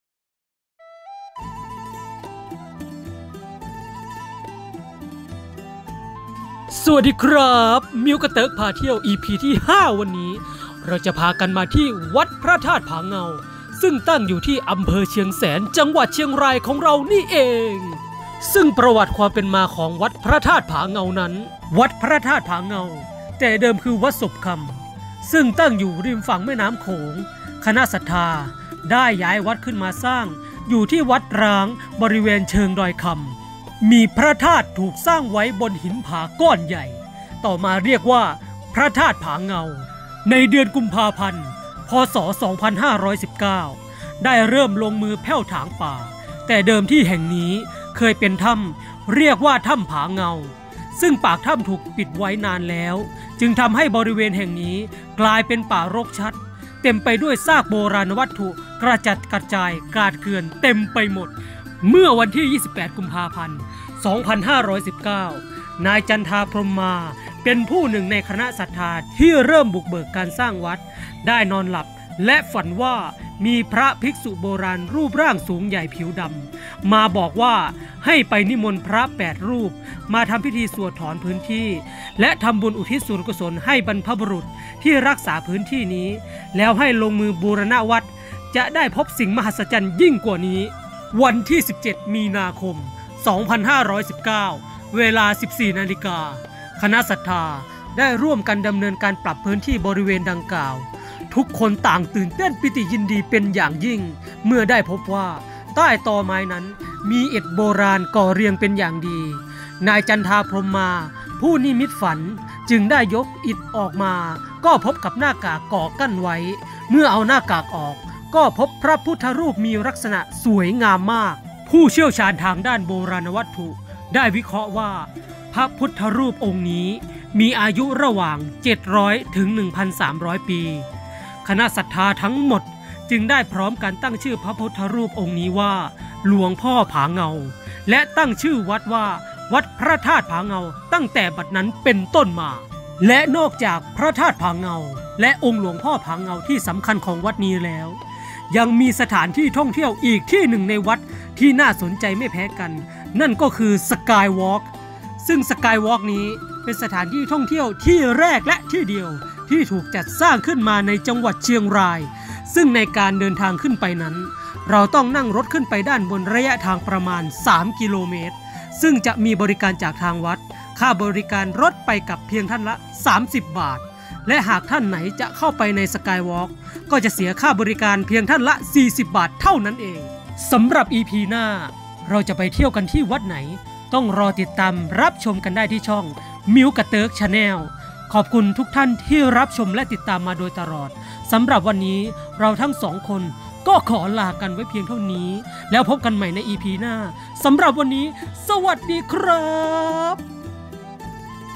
มิ้วกะเติ๊กพาเที่ยว ep 5 วัดพระธาตุผาเงา อ.เชียงแสน จ.เชียงราย
ประวัติวัดพระธาตุผาเงา
         จากพงศาวดารโยนกบอกว่า ขุนผาพิงหรือขุนพิง (พระองค์พิง) ผู้ครองนครโยนก องค์ที่ 23 ช่วงปี พ.ศ. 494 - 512 เป็นผู้สร้างเจดีย์ไว้บนหินก้อนใหญ่ที่เชิงเขาดอยจันทร์ ซึ่งเข้าใจว่าหมายถึงพระธาตุผาเงาในวัดนี้ พระธาตุผาเงาได้ชำรุดทรุดโทรมลงมาก คาดว่าน่าจะมีการบูรณะมาบ้าง เพราะพระธาตุองค์นี้ตั้งอยู่ที่ลาดต่ำสุดของภูเขา ซึ่งง่ายต่อการดูแลซ่อมแซม
         ส่วนผู้สร้างพระธาตุจอมจันและพระธาตุเจ็ดยอด อยู่บน "ดอยคำ" และ "ดอยจัน" ตามลำดับ เมื่อขุนลังได้ขึ้นครองเมือง "เวียงเปิกสา" (เมืองเชียงแสนปัจจุบันนี้) ช่วงปี พ.ศ. 996-1007 พระองค์ได้ชักชวนไพร่บ้านชาวเมืองทั้งหลายให้ช่วยกันสร้างเจดีย์ไว้บนยอดดอยคำ ซึ่งปัจจุบันชาวบ้านเรียกว่า "ตอดจันทร์" เจดีย์ที่ว่านี้หมายถึงพระธาตุจอมจันและพระธาตุเจ็ดยอด เจดีย์ทั้ง 2 องค์นี้ถูกภัยธรรมชาติทำลาย เช่น ถูกแดด - ฝน และลมพัดทำลายมานับพันปีจนเหลือแต่ซากฐานไว้ประมาณ 5 เมตร ประกอบกับช่วยนั้นภาวะเศรษฐกิจอาจจะฝืดเคืองจนทำให้ชาวบ้านทอดทิ้งศาสนาขาดการดูแลเอาใจใส่และที่ฐานองค์พระเจดีย์ทั้งสองก็มีรอยขุดเจาะ อาจจะเป็นฝีมือของพวกนักสะสมของเก่าเสาะแสวงหาโบราณวัตถุก็เป็นได้

อ้างอิงข้อมูลจาก : วิกิพีเดีย